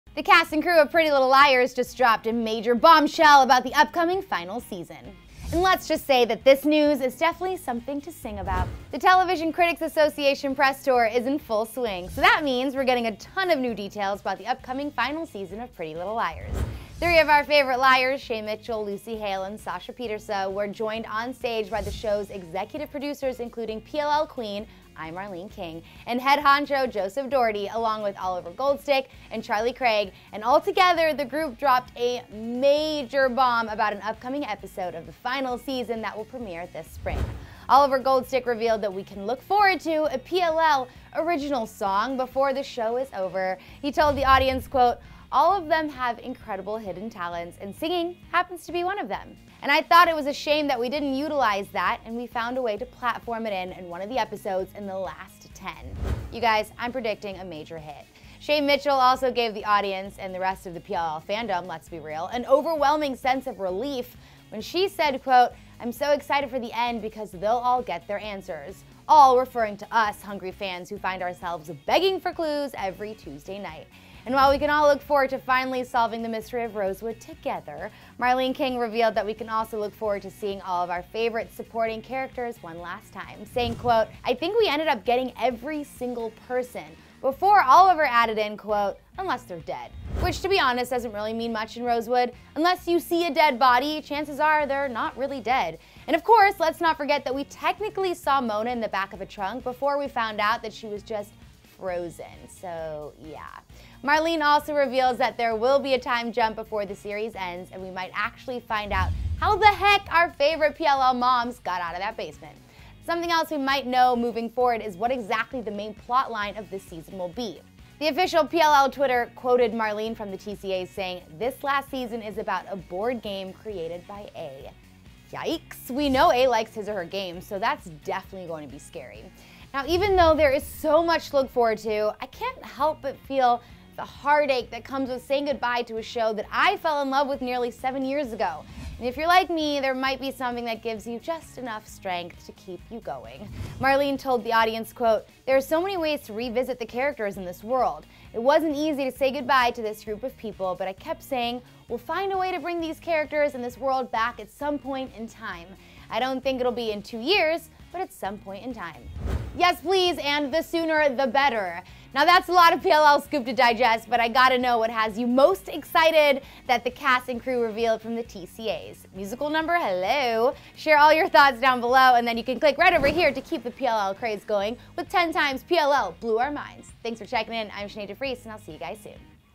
Pretty Little Liars Final Season Teases Original Song, Time Jump & Returning Faces
The cast & crew of Pretty Little Liars just dropped a major bombshell about the upcoming final season
And let’s just say this news is definitely something to sing about
The Television Critics Association press tour is in full swing, so that means we’re getting a ton of new details about the upcoming final season of Pretty Little Liars.
Three of our favorite liars, Shay Mitchell, Lucy Hale, and Sasha Pieterse were joined onstage by the show’s executive producers including PLL Queen, I. Marlene King, and head honcho, Joseph Dougherty, along with Oliver Goldstick and Charlie Craig, and altogether, the group dropped a major bomb about an upcoming episode of the final season that will premiere this Spring.
Oliver Goldstick revealed that we can look forward to a PLL original song before the show is over. He told the audience QUOTE “All of them have incredible hidden talents and singing happens to be one of them. And I thought it was a shame that we didn’t utilize that, and we found a way to platform it in one of the episodes in the last 10.”
I’m predicting a major hit. Shay Mitchell also gave the audience, and the rest of the PLL fandom, let’s be real, an overwhelming sense of relief when she said QUOTE “I’m so excited for the end, because they’ll all get their anwsers.” All, referring to us hungry fans who find ourselves begging for clues every Tuesday night. And while we can all look forward to finally solving the mystery of Rosewood together, Marlene King revealed that we can also look forward to seeing all of favorite supporting characters one last time. Saying QUOTE “I think we ended up getting every single person,” before Oliver added in QUOTE “unless they’re dead.” Which, to be honest, doesn’t really mean much in Rosewood. Unless you see a dead body, chances are they’re not really dead, and of course let’s not forget that we technically say Mona in the back of a trunk before we found out that she was just “frozen” soooo…. Yeah.
Marlene also reveals that there will be a time jump before the series ends, and we might actually find out how the heck our favorite PLL moms got out of that basement. Something else we also might know moving forward is what exactly the main plotline of the season will be. The official PLL Twitter quoted Marlene from the TCA’s saying “This last season is about a board game created by A.” YIKES. We know A likes his/her games, so that’s definitely going to be scary.
Now, even though is so much to look forward to, I can’t help but feel the heartache that comes with saying goodbye to a show I fell in love with nearly 7 years ago, and if you’re like me, there might be something that gives you enough strength to keep on going. Marlene told the audience QUOTE “There are so many ways to revisit characters and world. It wasn’t easy to say goodbye to this group of people but I kept saying, ‘We’ll find a way to bring these characters and this world back at some point in time.’ I don’t think it’ll be in two years, but at some point in time.”
Yes, please! And the sooner, the better. Now that’s a lot of PLL scoop to digest, but I gotta know what has you most excited that cast and crew revealed from the TCA’s. Musical Number, hello? Thanks for checking in, I’m Sinead de Vries, and I’ll see you guys soon!